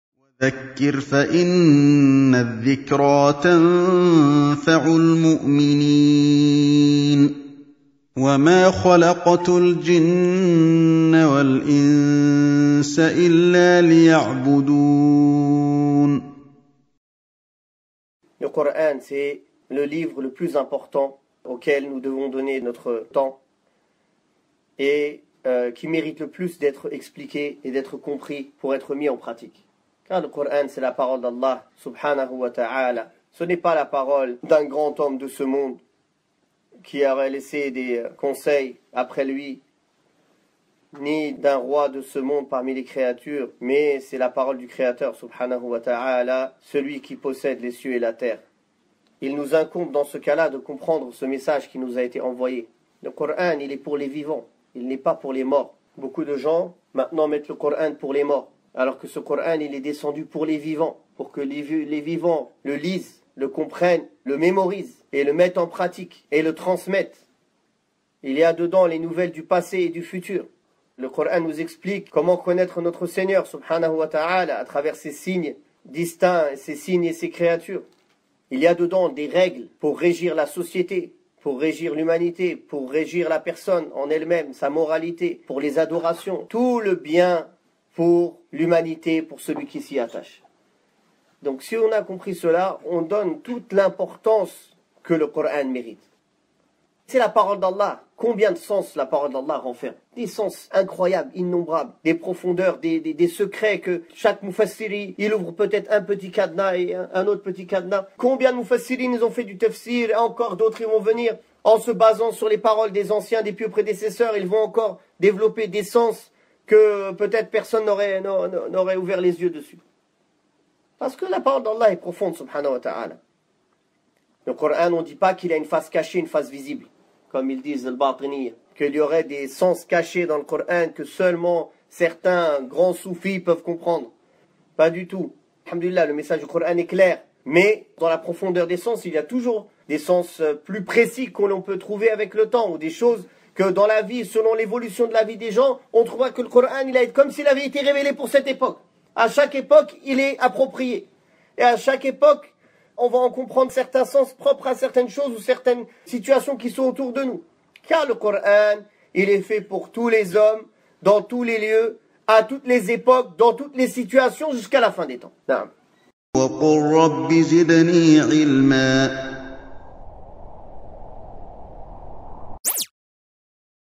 Le Qur'an (Abou Laïth 'Othmân Al Armany) - Dourous-Sounnah.com
Extrait_de_cours # 138: Le Qur'an

Tafsir 032 Juzz 'Amma Al Bouroudj (les constellations)
(2014)
Sourate 085